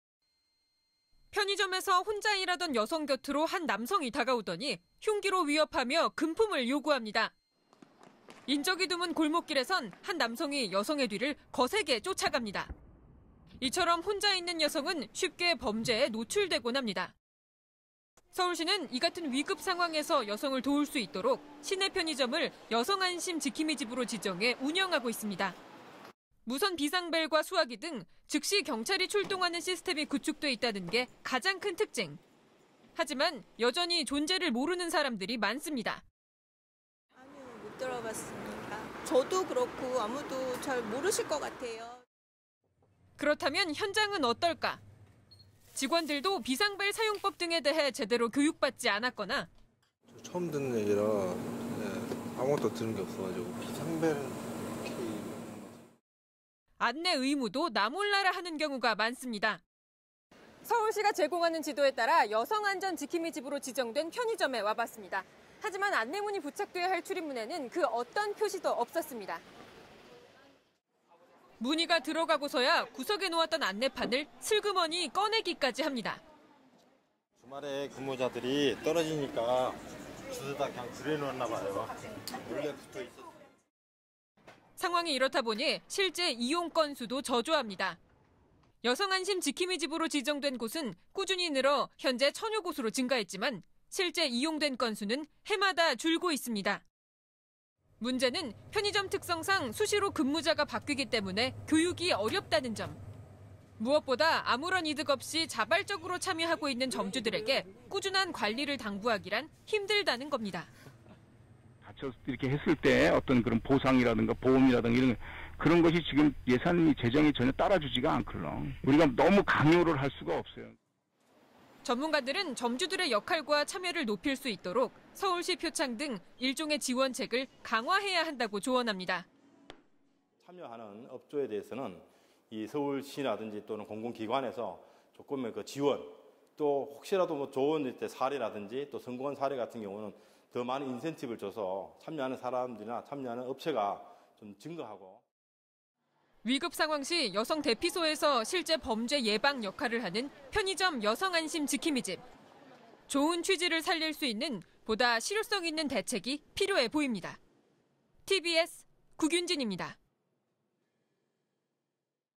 [tbs] 편의점 ‘여성안심지킴이집’…실효성 높이려면?
늦은 밤 혼자 귀가하거나 일할 때 불안해하셨던 여성분들 많으시죠. 이 같은 여성들을 위해 서울시가 일부 편의점을 여성 보호 공간으로 운영하고 있는데요. 시행 4년째이지만 아직 잘 모르는 시민들이 많고 이렇다 보니 이용률도 저조해 주먹구구식으로 운영되는 경우가 많았습니다.